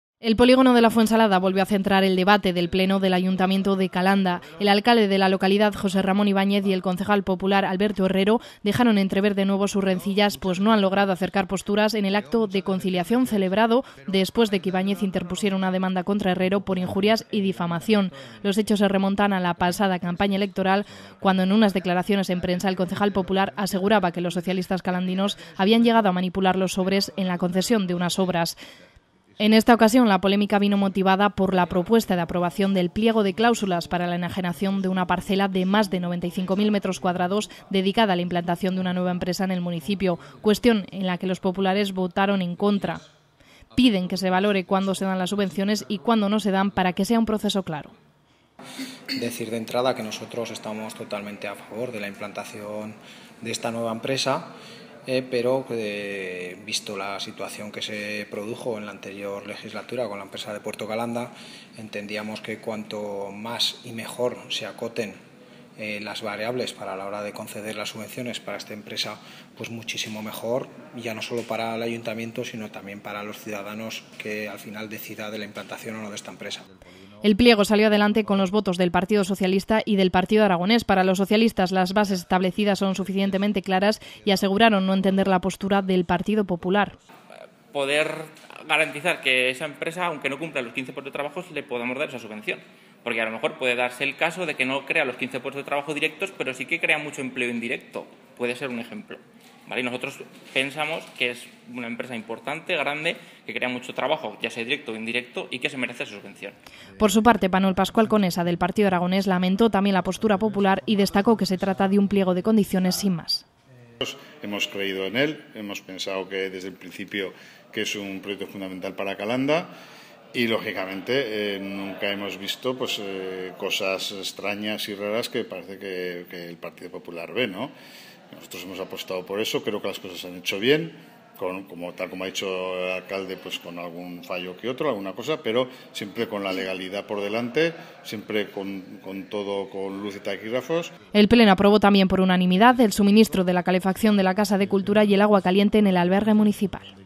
PLENO CALANDA 29 julio 2015
El debate del último pleno celebrado en Calanda se centró en el polígono de La Fuensalada.